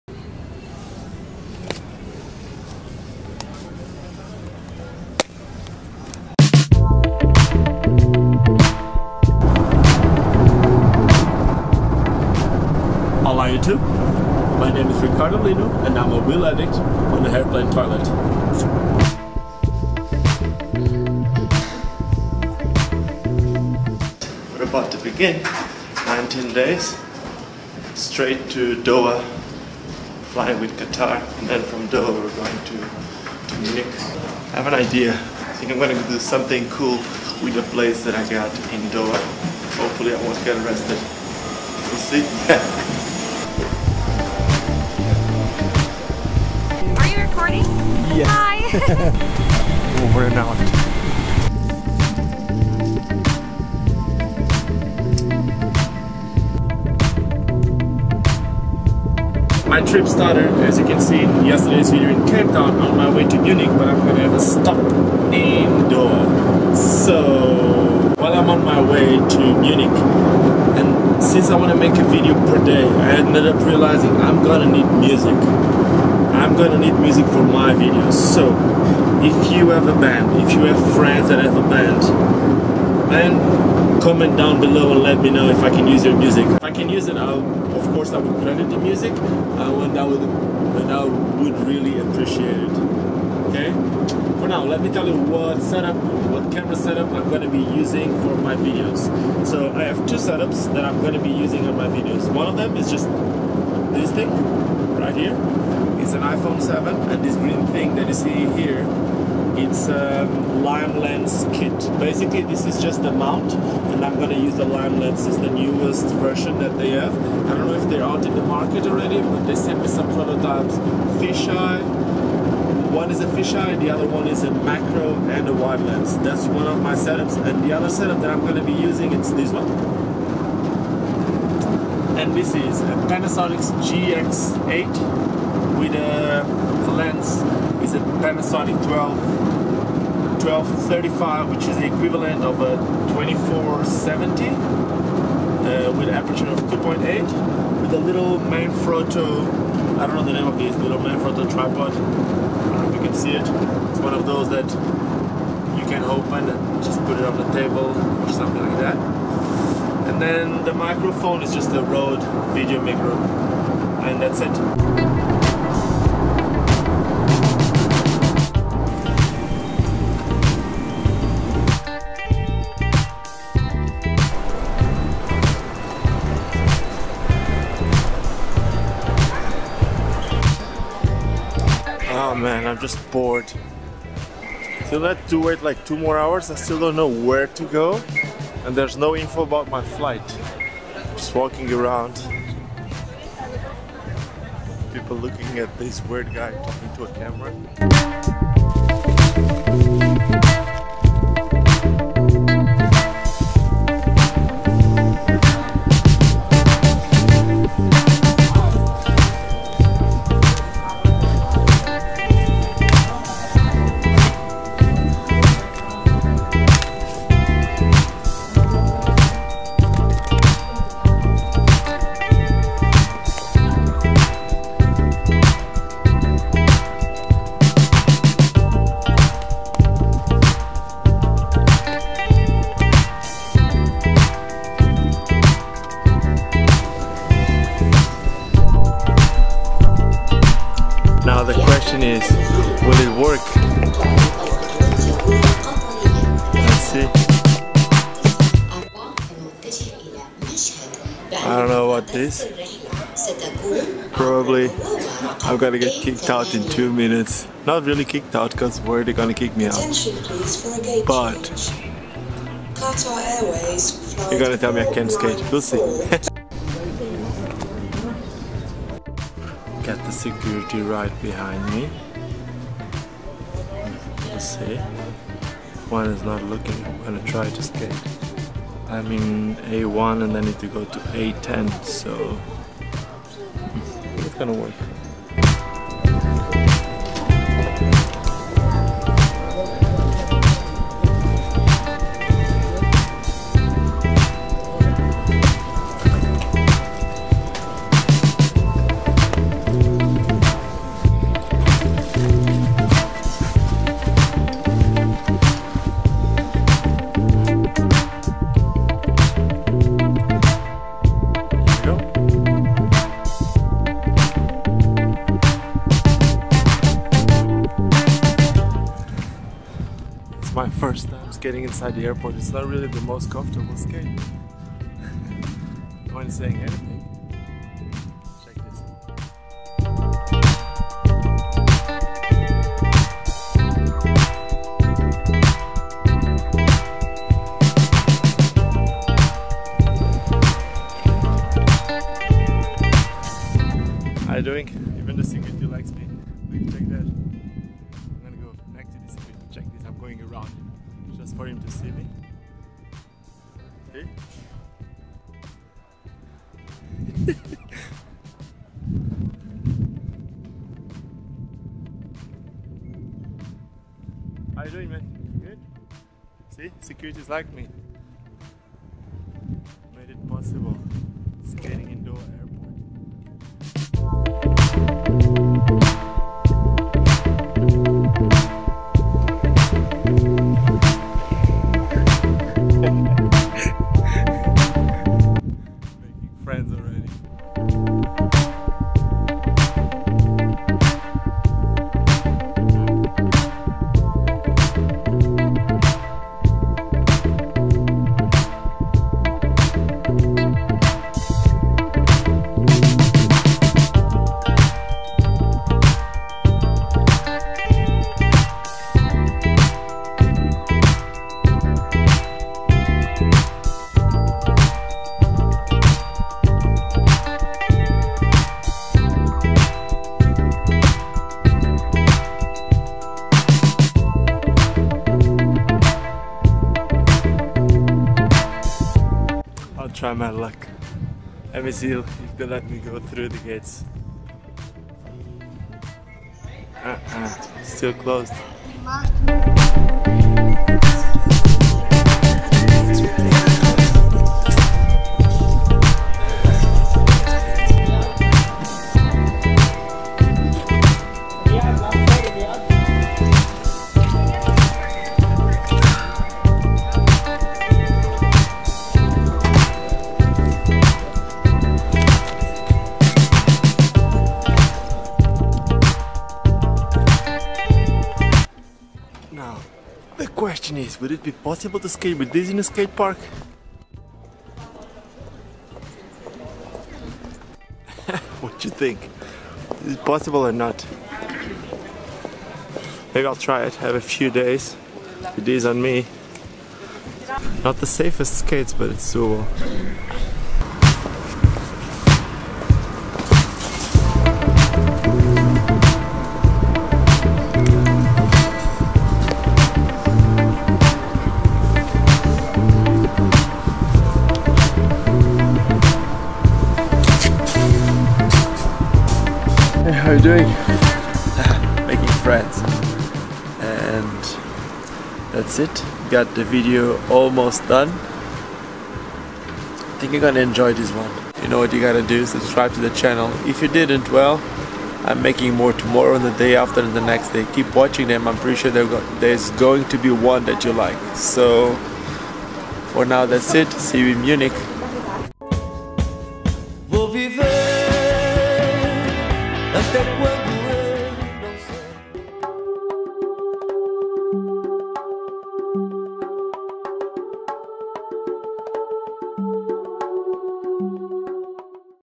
SKATED INSIDE THE AIRPORT WITH THESE DIY SKATES // VLOG26
► On my way to the first stop on my european trip had a "pit stop" in Doha, Qatar, and since all i had was my roller skates plates, i ended up lacing them up to my shoes and cruise around the airport

then i edited the video real quick...but not quick enough to ALMOST LOOSE MY NEXT FLIGHT

Camera Gear used in my videos:

- VLOG camera
- MAIN Camera
- ACTION CAMERA